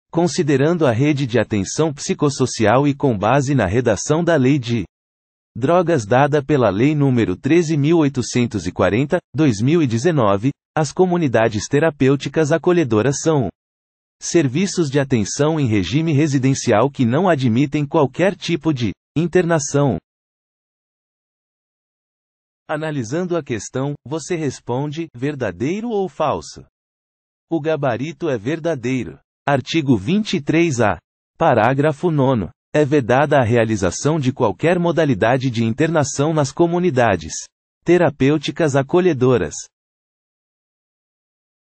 QScon Direito: [Art. 23-A, § 9º, Lei 11.343/2006] FCC 2021 - Defensor Público (DPE-AM)
Art. 23A, § 9º, Lei 11.343/2006 | FCC 2021 | Defensor Público | (DPEAM)

Considerando a Rede de Atenção Psicossocial e com base na redação da Lei de. Drogas dada pela Lei número 13.840/2019, as comunidades terapêuticas acolhedoras são. serviços de atenção em regime residencial que não admitem qualquer tipo de. internação.

Gabarito: Verdadeiro